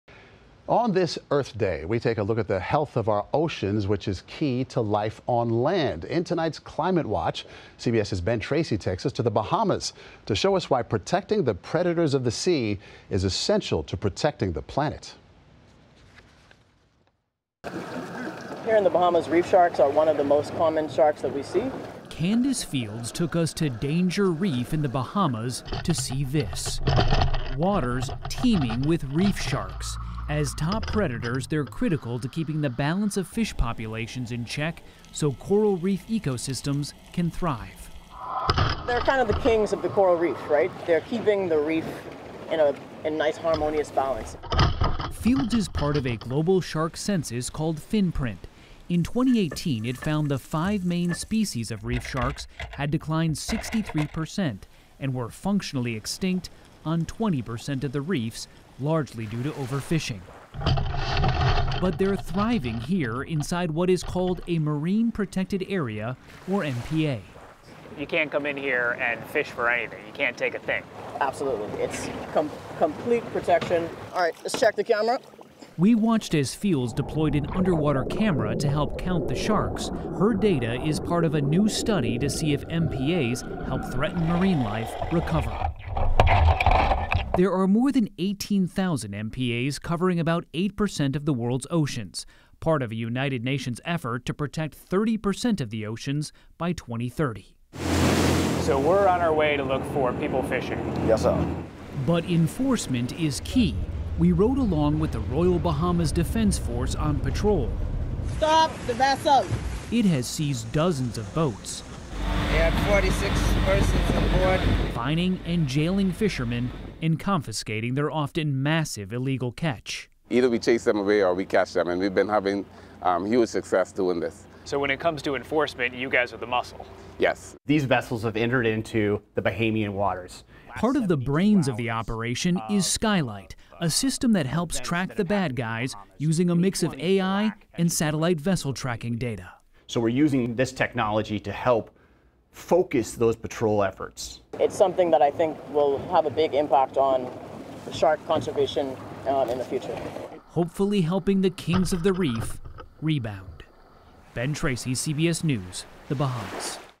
Scientists work to save reef sharks to help keep marine life balance
A global shark census found in 2018 that the population of five main reef shark species, which are key to balancing the ecosystem of coral reefs, had declined 63%. Ben Tracy takes a look at a group working to protect these necessary predators.